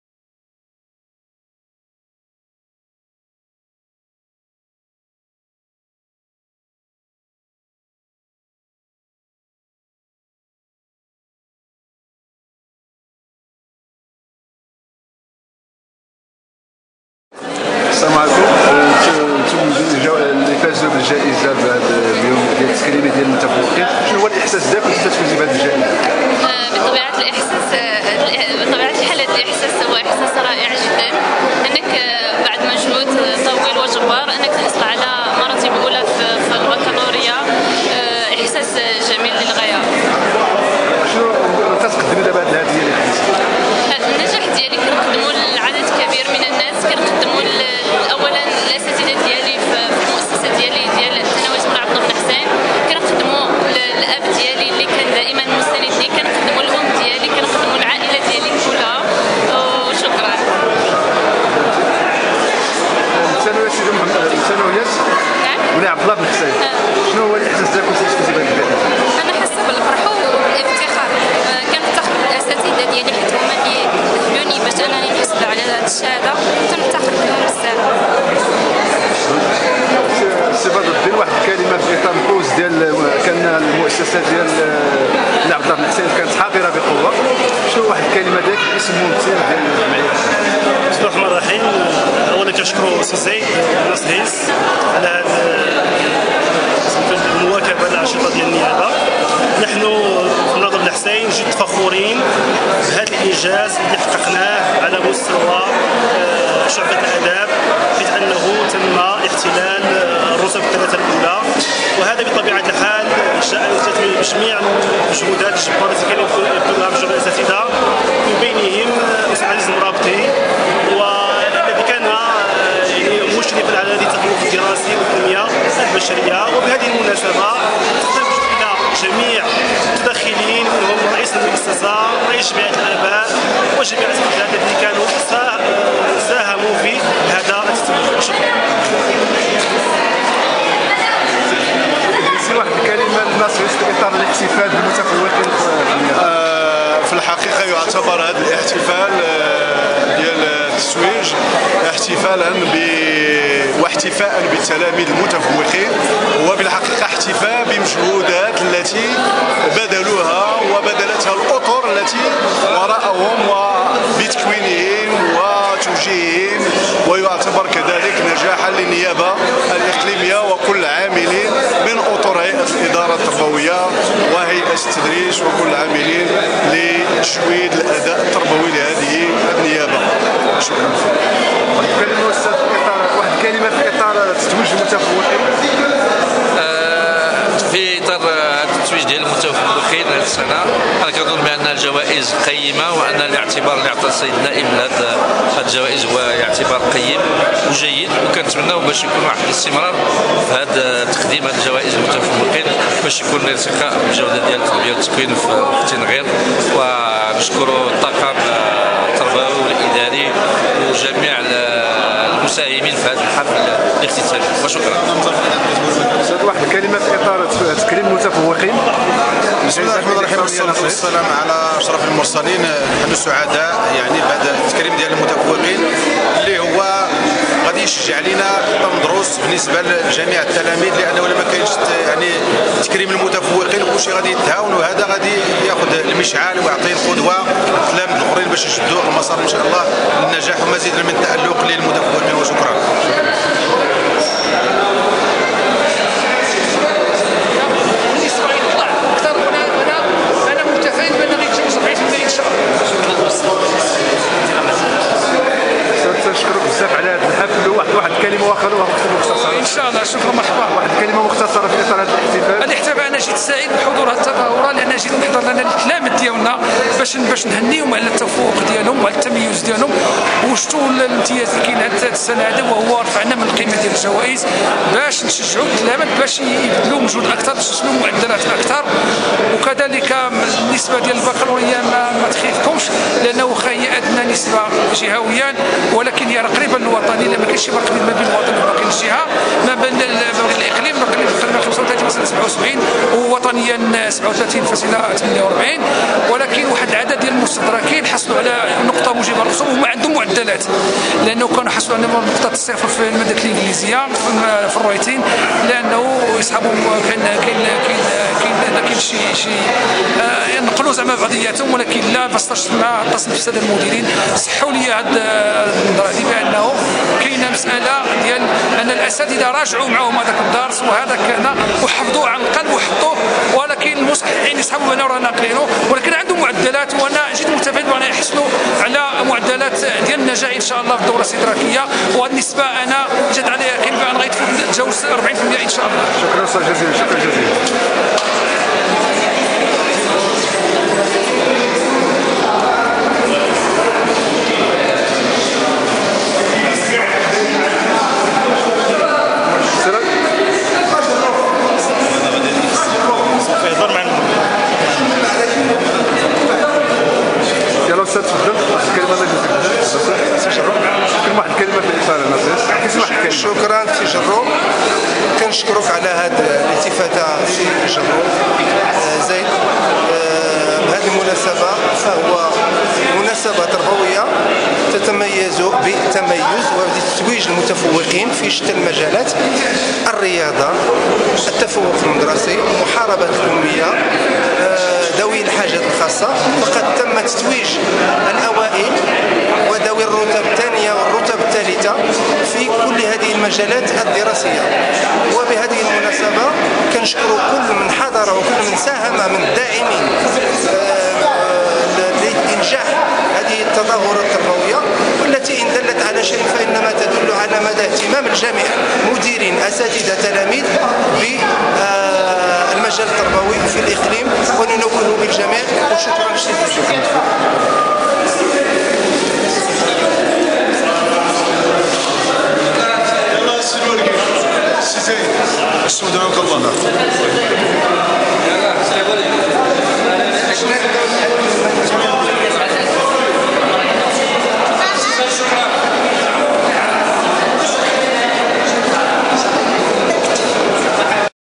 تنغير: استطلاع حول تتويج المتفوقين دراسيا 2013
بعد حفل تتويج المتفوقين دراسيا بدار الثقافة بقلعة مكونة 05/07/2013  مساء استقت ناس هيس الاستطلاعات التالية
ربورتاج : زايد جرو